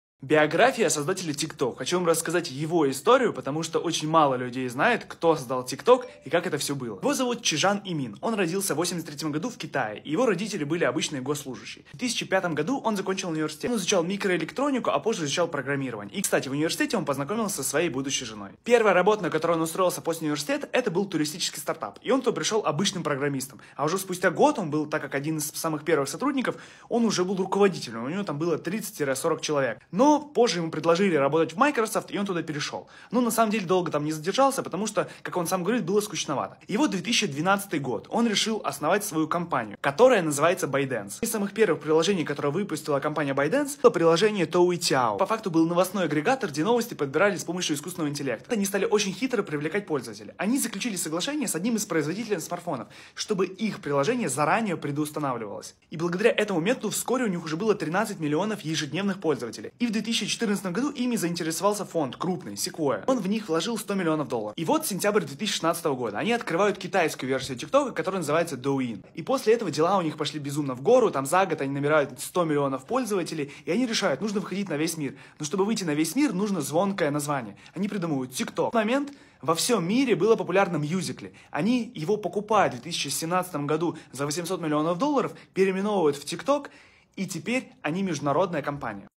Биография создателя ТИК ТОКА
В марте 2012 года инженер-программист Чжан Имин, окончивший Нанькайский университет в Тяньцзине, основал интернет-компанию ByteDance, которая в августе 2012 года запустила приложение Toutiao. В сентябре 2016 года на китайском рынке появился видео-сервис Douyin. Летом 2017 года видео-сервис TikTok был запущен для iOS и Android на большинстве рынков за пределами Китая. В ноябре 2017 году ByteDance приобрела китайско-американский видеохостинг musical.ly, который в августе 2018 года объединился с TikTok (тогда же бренд TikTok вышел на рынок США). Если в 2017 году ByteDance была оценена в 20 млрд долл., то в 2018 году - уже в 75 млрд долл.